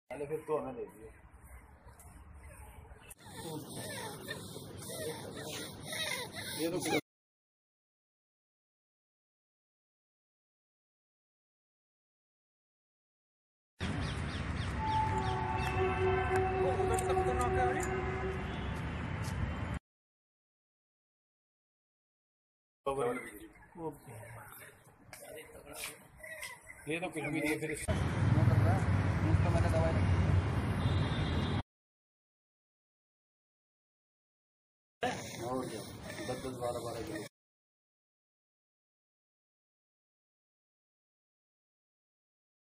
Indian Palm Civet
in a rare occurrence, Asian palm Civet caught alive in human habitat

animal that eats dead bodies from graves ,
The Asian palm civet (Paradoxurus hermaphroditus, common palm civet, toddy cat, musang
kabar biju kabbar bijju

kalawedda